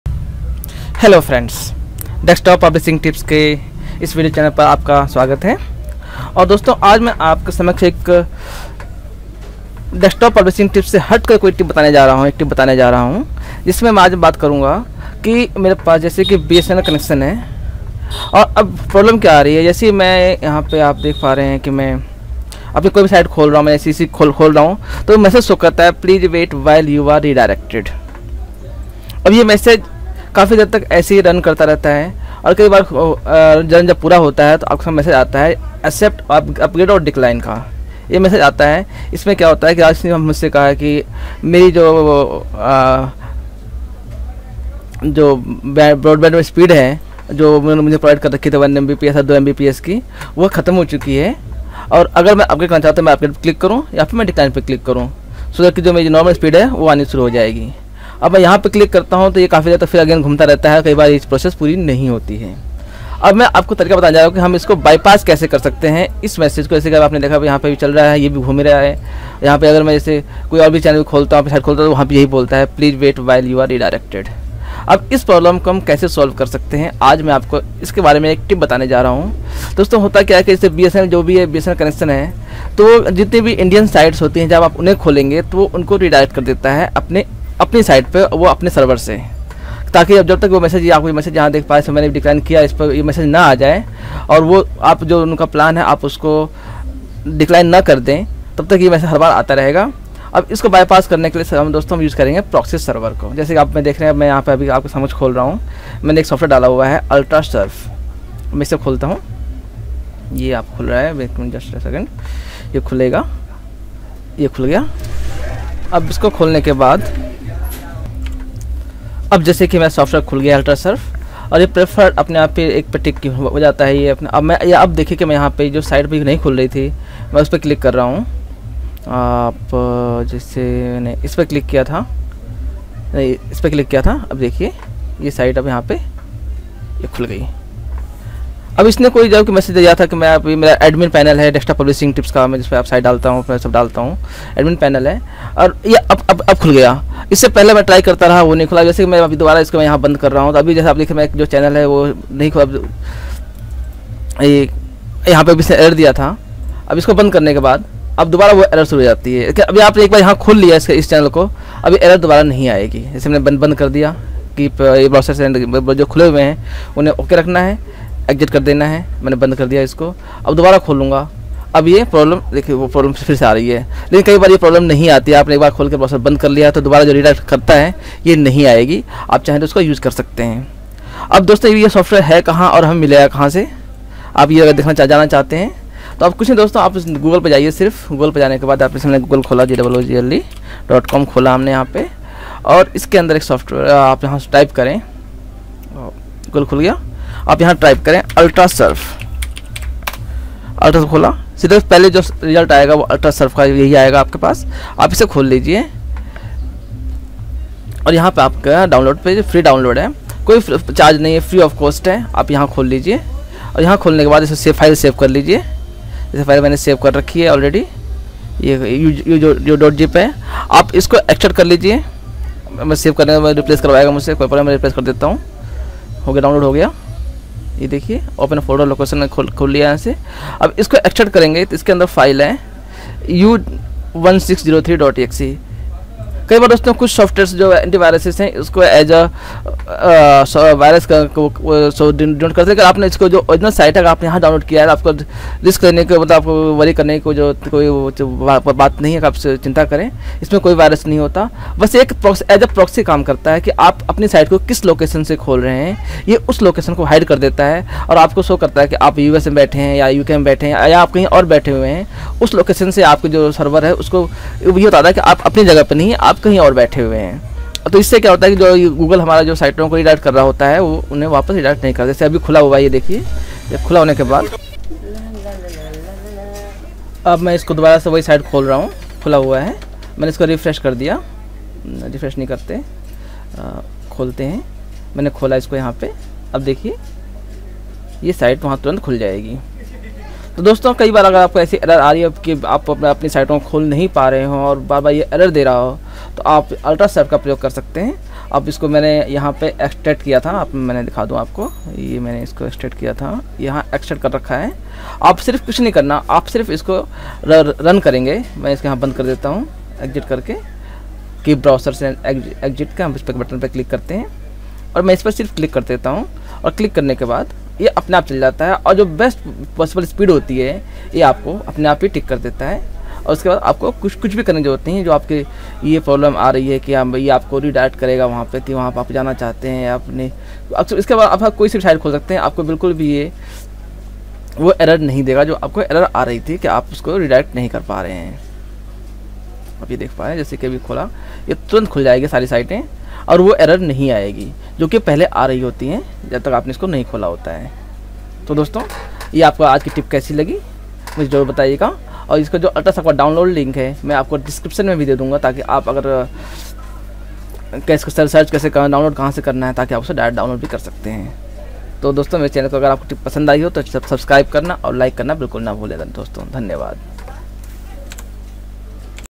Solved! Please wait while you are redirecting....BSNL Error HIndi Video
If you are getting this message..
Please wait whiel you are redirecting...

As you are a premium Broadband user of BSNL,We would like to inform that you have exhausted your mandated broadband speed (as per plan chosen by you).
We want you to continue browsing at higher speed.
Please click on "Upgrade" option as per your convenience.

Then this tip will surely solve your problem...